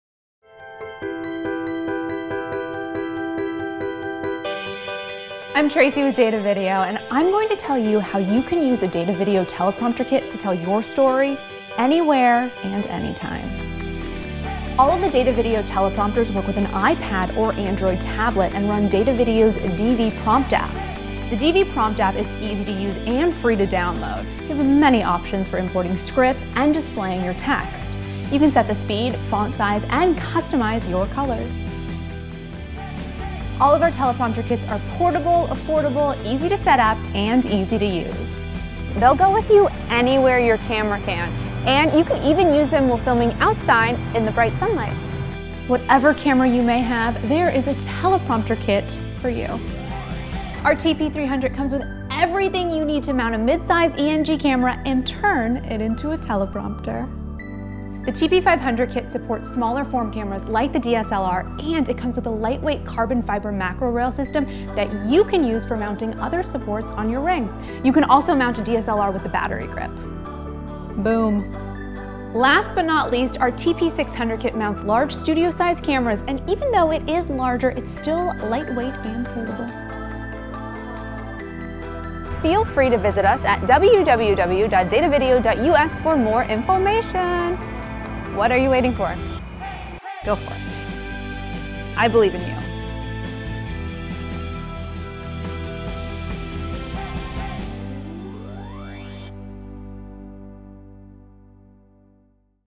Datavideo Bluetooth Teleprompter Kits for Apple iPad and Android Tablets
Discover affordable teleprompting solutions for use with DSLR cameras, professional ENG cameras, and handheld video cams.

Turn your Apple iPad or Android Tablet into a professional teleprompting system with a Datavideo teleprompter kit. Our teleprompter kits turn virtually any type of camera into fully functioning professional teleprompters for your video production. It’s the perfect solution for giving talent an easy way to read scripts for news, sports, entertainment, and many other types of shows.

You can easily load scripts, adjust speed, character size, and colors. The wired remote or bluetooth wireless remote let you control speed and direction for more professional results. The TP-300, TP-500, and TP-600 works with all models of the iPad, including the latest Retina Display version.